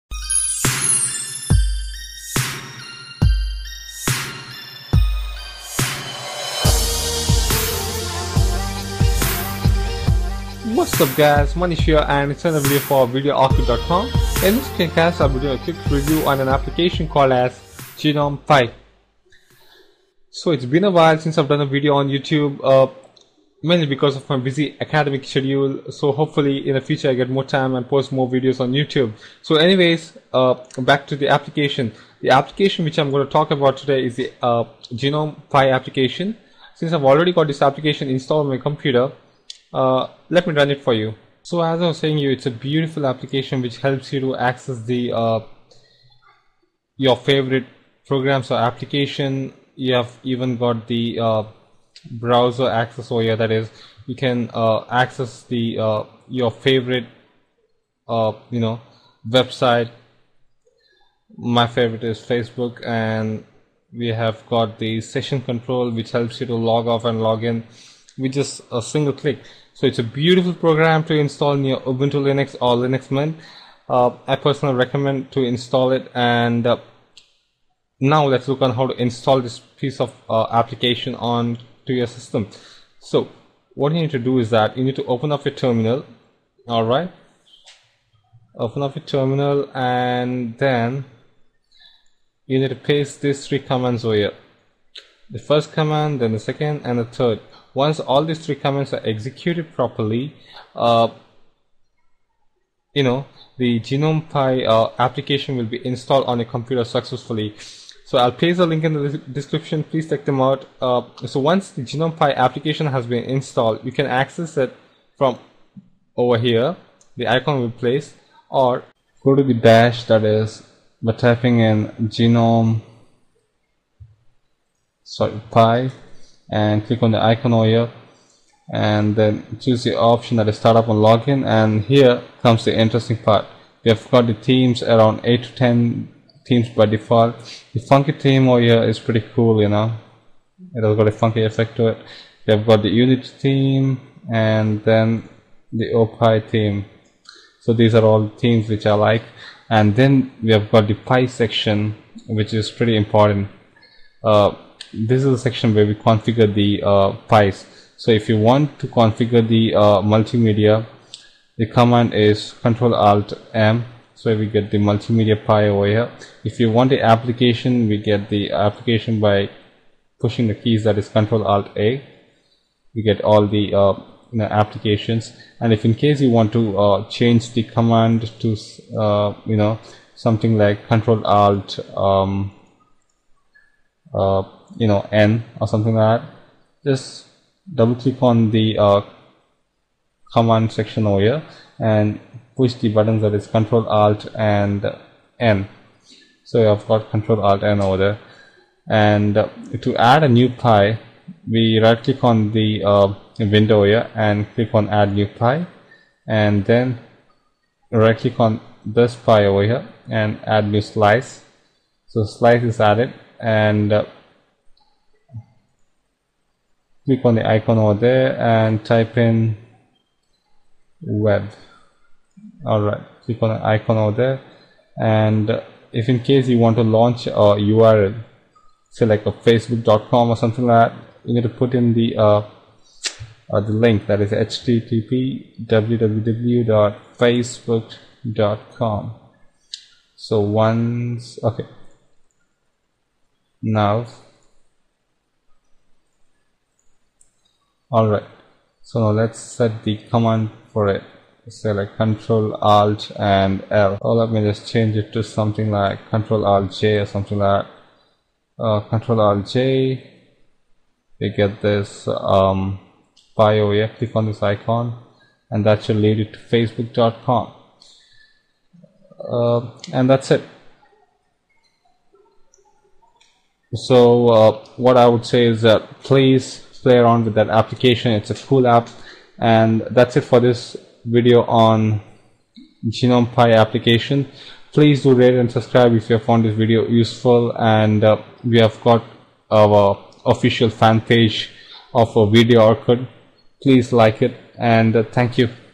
GNOME Pie -- Cool Application Launcher for Linux
GNOME Pie is a keyboard based application launcher which works on Ubuntu 11.04 Natty Narwhal,Ubuntu 11.10 Oneiric Ocelot and Linux Mint.You press a key combination (which is fully editable) and an animated circular launcher -- called a Pie -- appears. Using your mouse you can launch an application by clicking on a button or shortcut within the 'Pie'. Shortcuts are called 'Slices'.

Gnome application can do more than just launching application,it can be used for logging in/out/restart/access your favorite website and much much more.You can use this application in Ubuntu and in Linux Mint to make your OS very cool and the best part about the application is,its user-friendly.

Instructions to install Gnome Pie

sudo add-apt-repository ppa:simonschneegans/testing
sudo apt-get update
sudo apt-get install gnome-pie